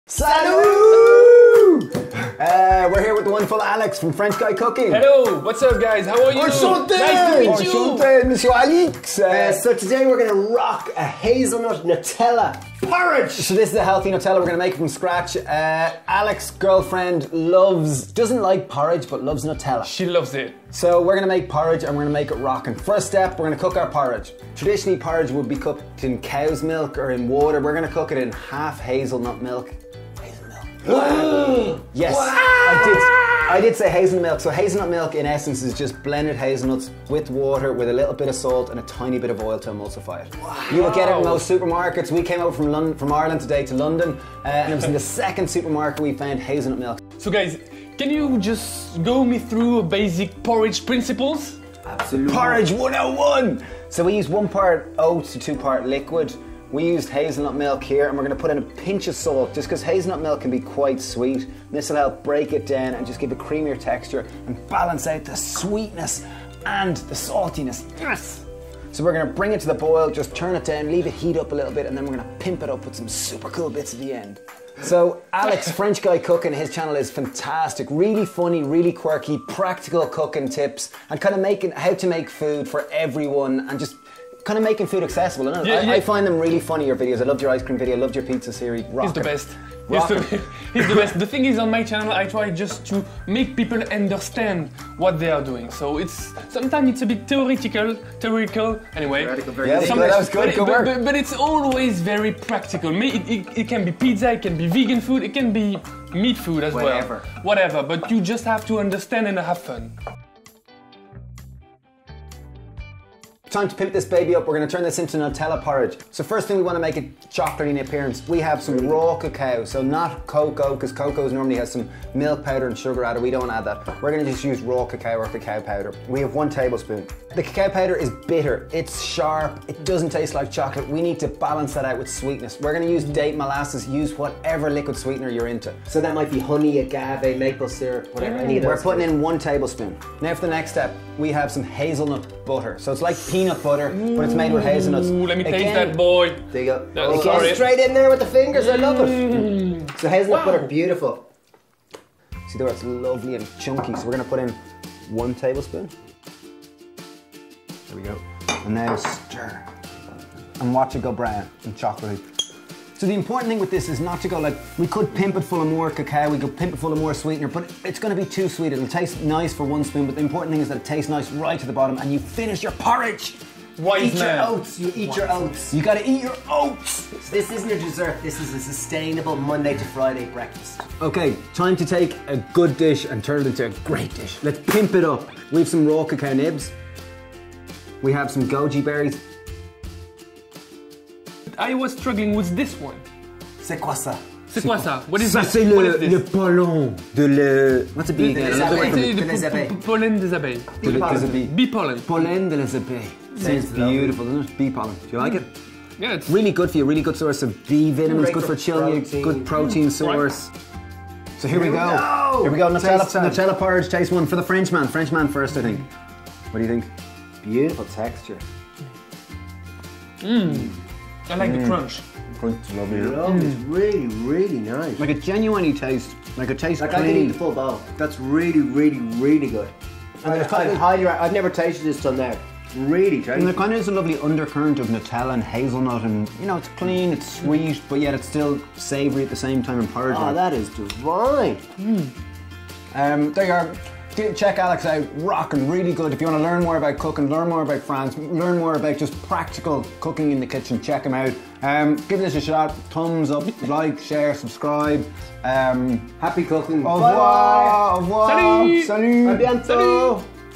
This porridge tastes like NUTELLA w/ Alex French Guy Cooking
We got to hang out and cook with the amazing Alex French Guy Cooking and we tried to bridge the gap between his love for Porridge and his Wife's love for Nutella.

Dave & Steve.